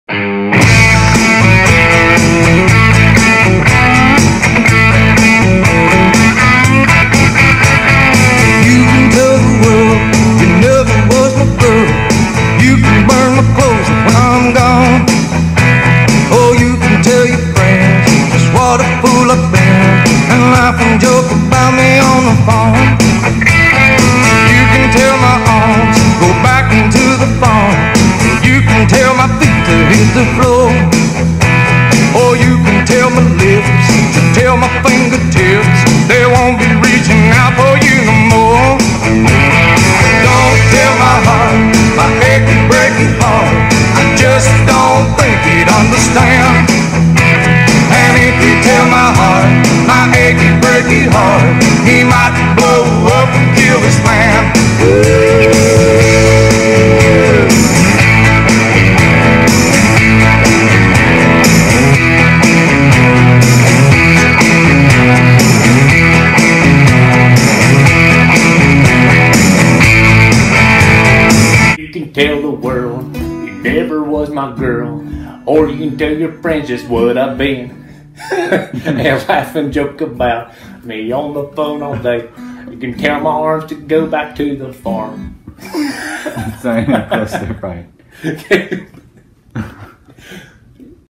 My achy breaky foot
It takes more than a broken foot and crutches to hold back his moves.